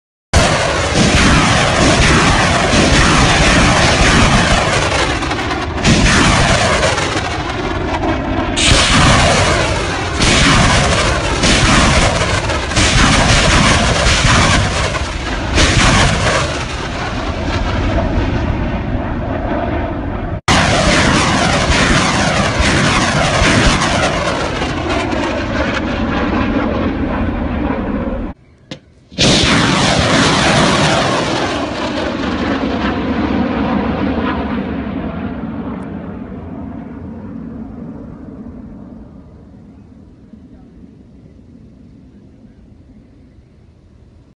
🇸🇦🐪 Saudi Royal Army & 🇾🇪🇸🇦 Hadi Loyalist Forces firing at 🇾🇪🇮🇷 Houthi Positions in Yemen.
🇸🇦❌🇾🇪📹 — Also on February 4th, 2022, footages, shared by local observers and netizens, show what appears to be 🇧🇷🐆 Brazilian-made Astros II MLRS belonging to 🇸🇦🐪 Saudi Royal Army & 🇾🇪🇸🇦 Hadi Loyalist Forces firing at 🇾🇪🇮🇷 Houthi Positions in Al-Hajjah, Northwestern Yemen.